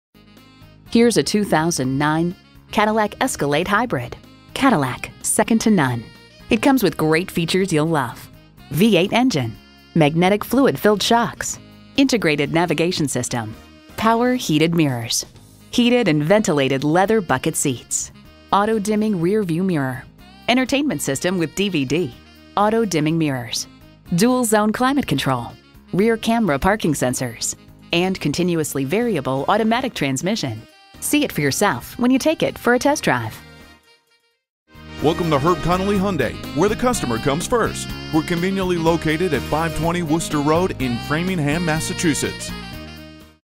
Used 2009 Cadillac Escalade Framingham, MA H15893B - SOLD
Year: 2009
Make: Cadillac
Model: Escalade
Trim: Hybrid
Engine: Vortec 6.0L SFI LIVC Hybrid
Transmission: 4-Speed eCVT
Mileage: 134371
Stock #: H15893B
VIN: 1GYFK43509R192655

Address:
520 Worcester Rd - Rte 9 East